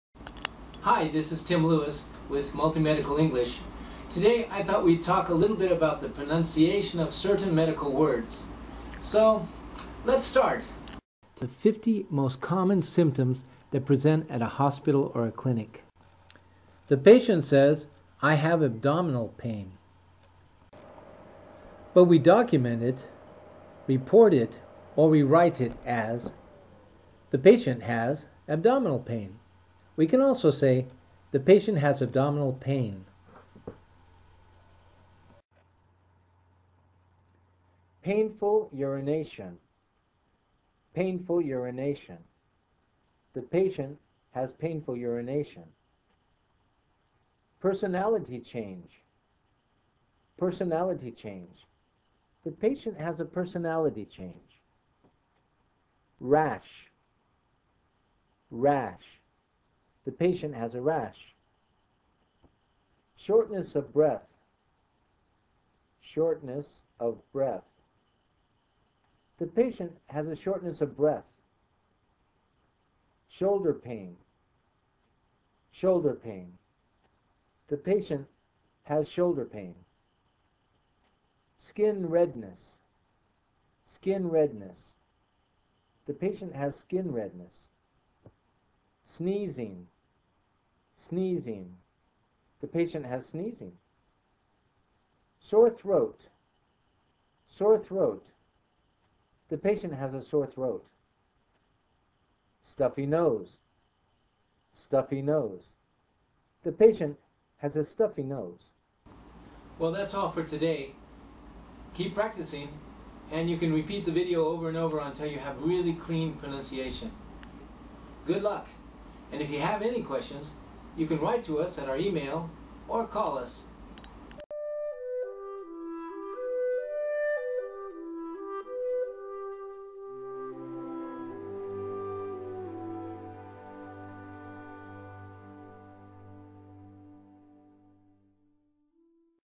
Pronunciation of 50 common symptoms (painful urination - stuffy nose) # 4 SEP
# 4  in a series - USMLE 2 CS Spoken English Proficiency:
  Mejora tu pronunciación en Inglés médico
Help practicing the pronunciation of the 50 most common symptoms in a Hospital or Clinic.  (painful urination - stuffy nose)
Skype : multimedical.english